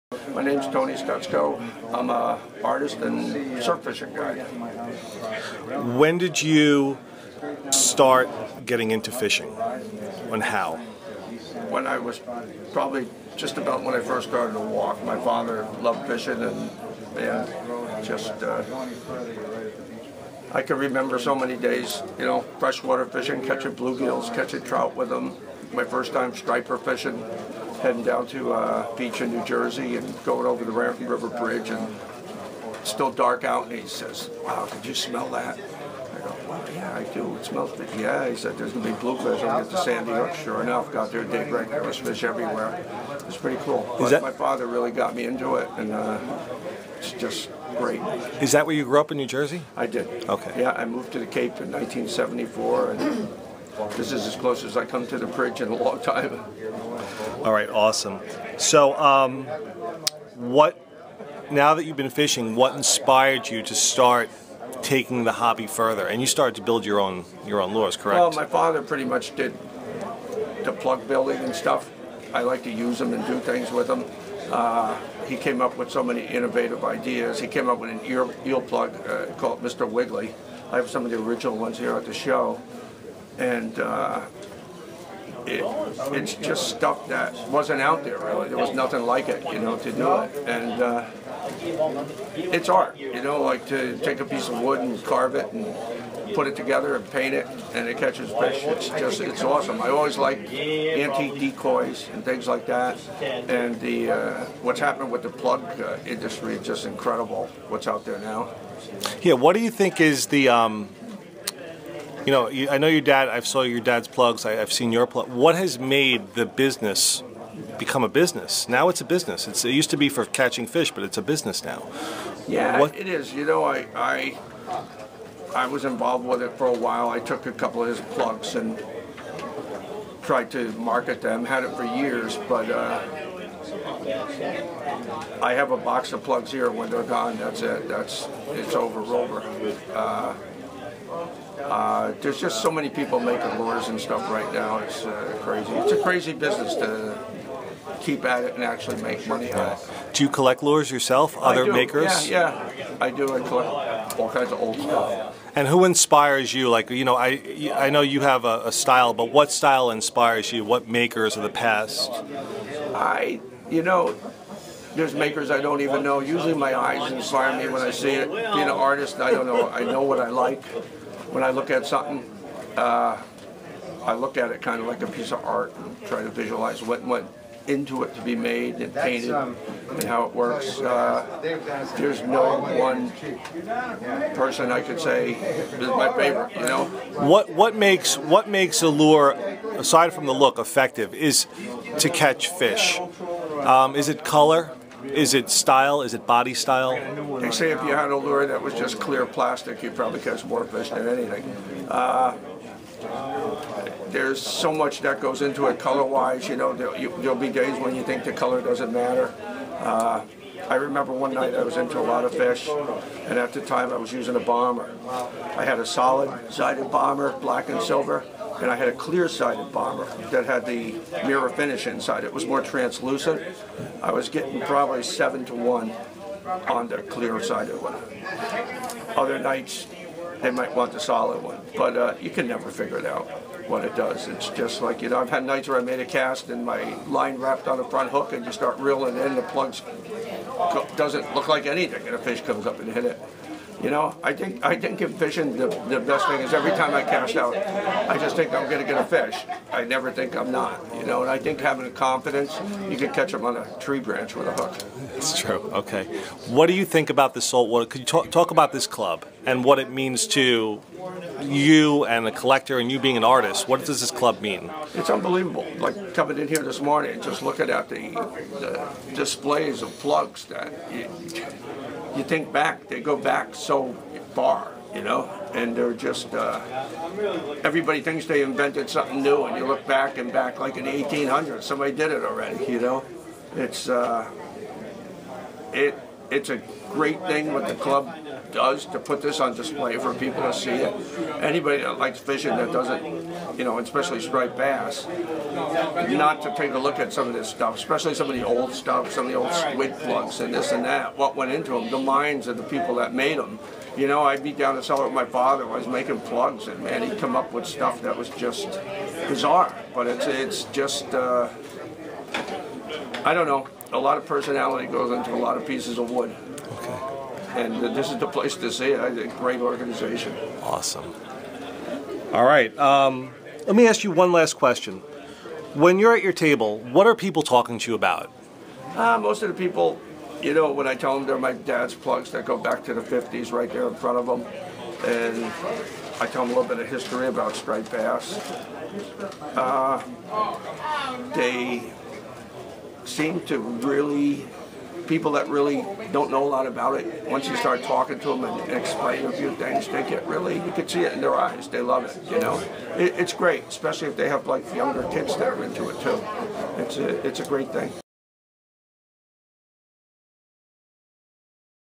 Tony Stetzko interview
This is an interview I did at the SWLCC convention in 2013 with legendary angler, former IGFA world record holder, saltwater guide and artist Tony Stetzko.